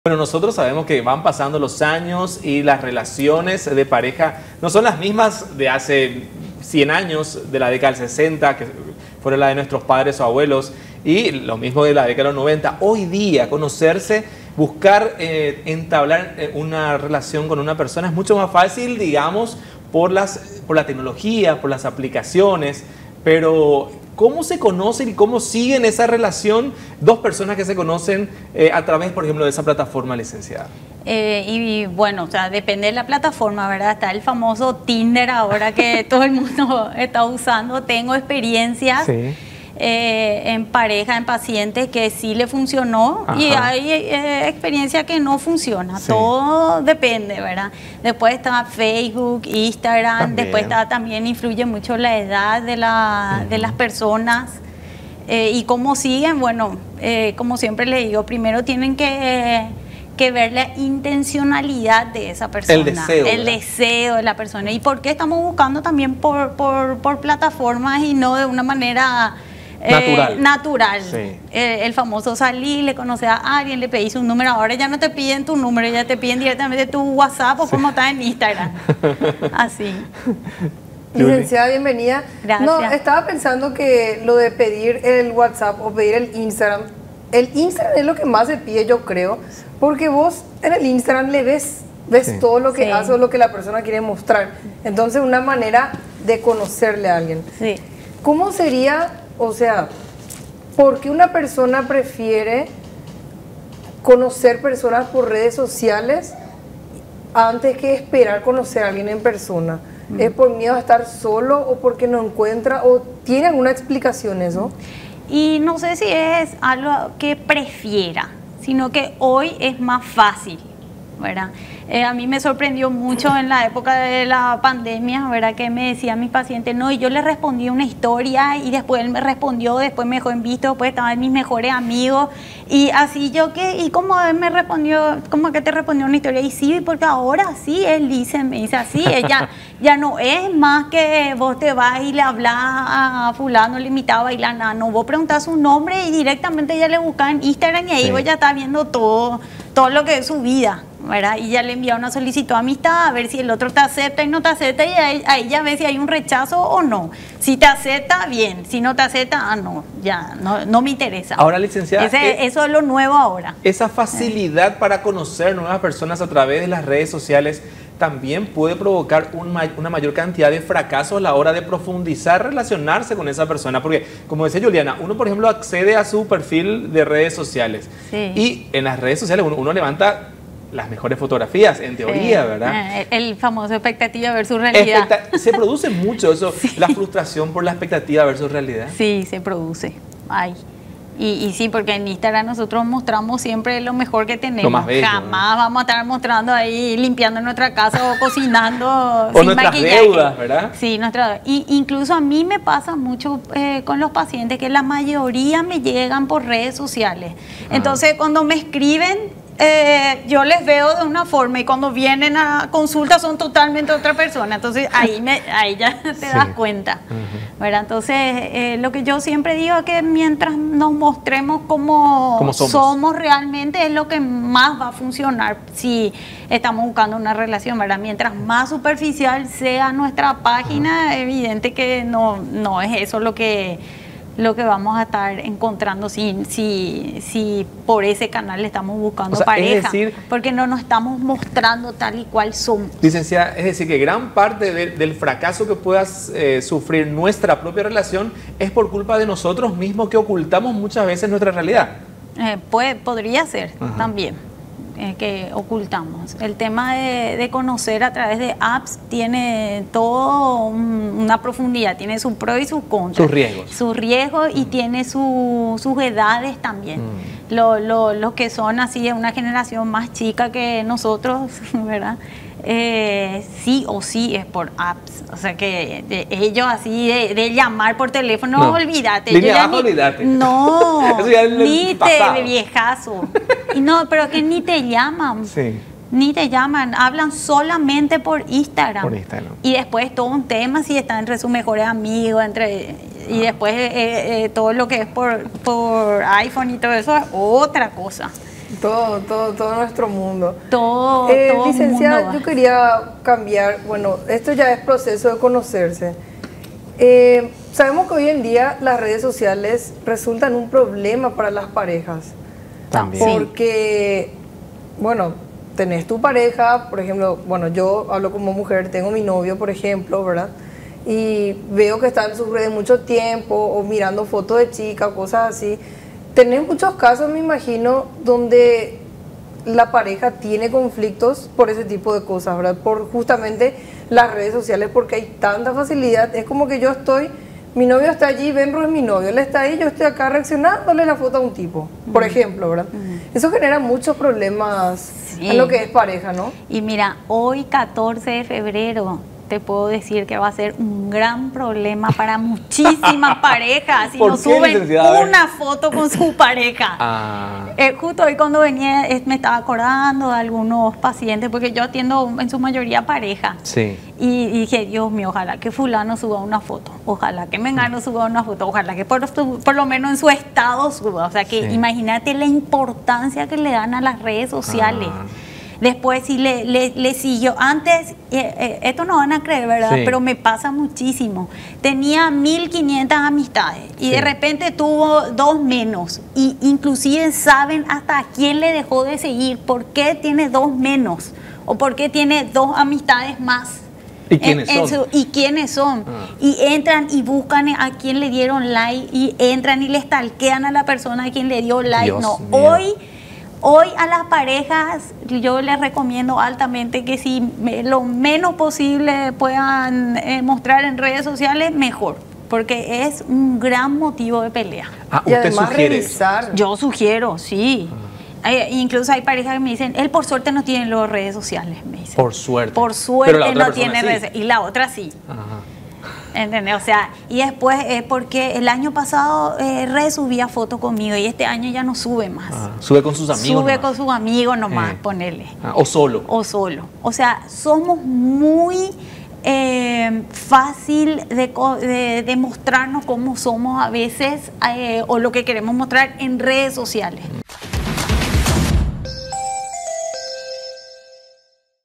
¿Cómo son las relaciones de parejas actualmente?
Los tiempos, en definitiva, han cambiado, y esto también con relación a dónde encontrar el amor de nuestras vidas. Hoy ya es más que habitual recurrir a las aplicaciones de citas para elegir alguien a medida.